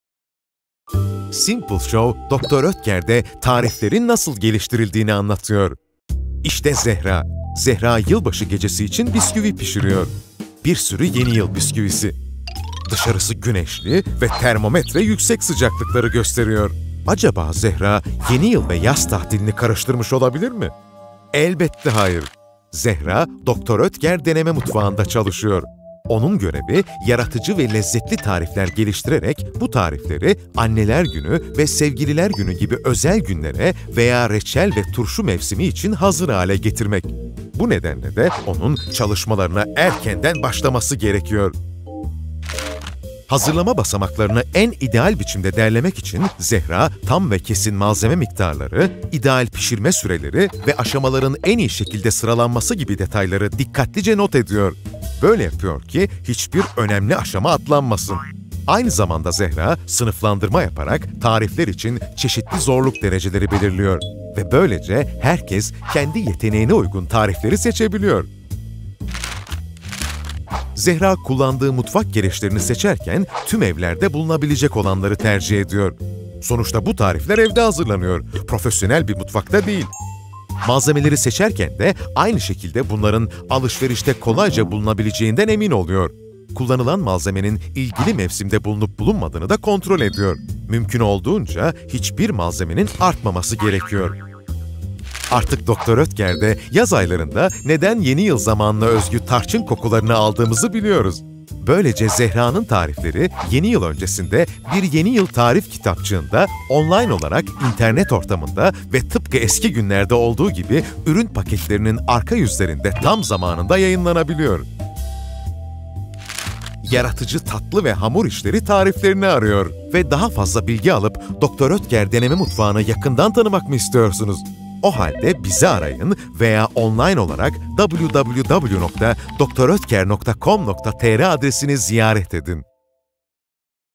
Dr. Oetker ile Tarif Geliştirme
Tarif geliştirme

Size, bir tarifin garantili bir şekilde nasıl geliştirildiğini ve yayınlandığını göstermek istiyoruz. Bunun için ilgili filmi izleyebilirsiniz!